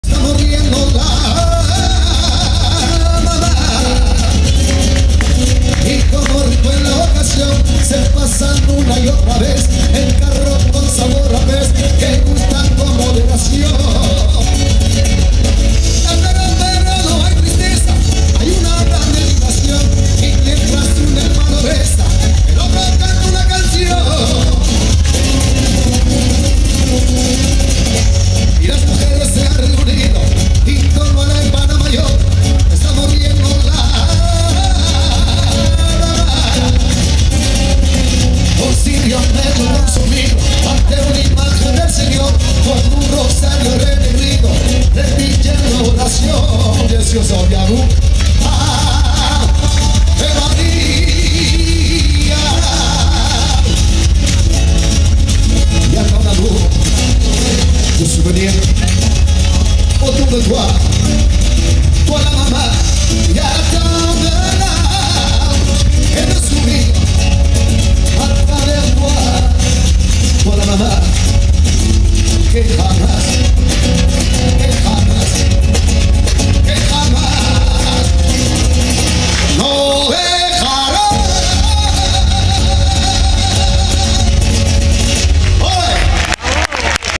Gerard Ferrer
A l'occasion d'une soirée en l'honneur de Manitas de Plata, à la Nouvelle Mairie de Montpellier